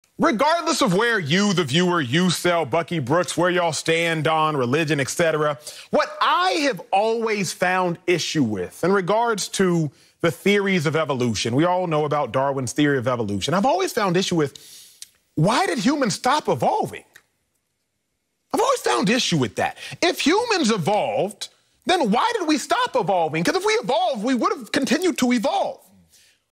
EMMANUEL ACHO EXPLAINS WHY SCIENTISTS ARE WRONG ABOUT EVOLUTION
@107round many experts believe in the Theory of Evolution but fmr. Eagles great Emmanuel Acho discovers a flaw in their theory.